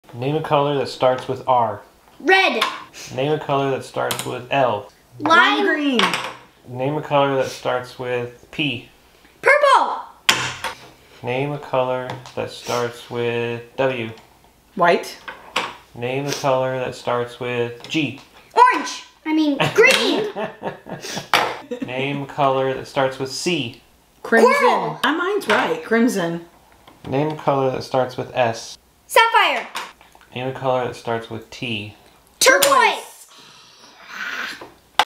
Color Trivia! TicTacToe!🤣
Tic Tac Toe Color Trivia with Mom vs Daughter! Play along with us and see who wins!

Game/Challenge Props:
Incohearent Family
Uno Cards